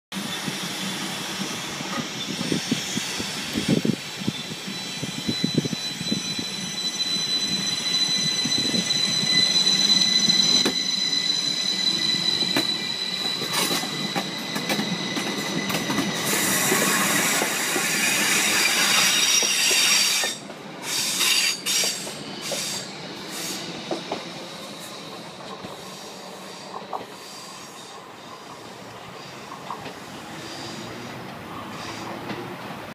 Northern Rail Class 333016 Leaving Shipley Station
This was filmed on the 8th June 2013